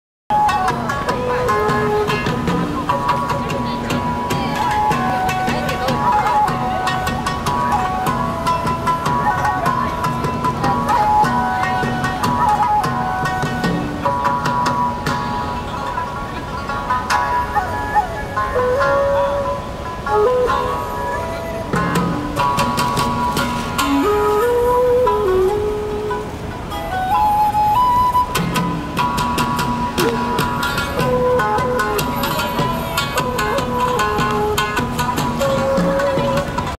Kyoto Fire Dancer 2
For a mere 100yen, we got a pretty good show. On the Kamogawa, no less!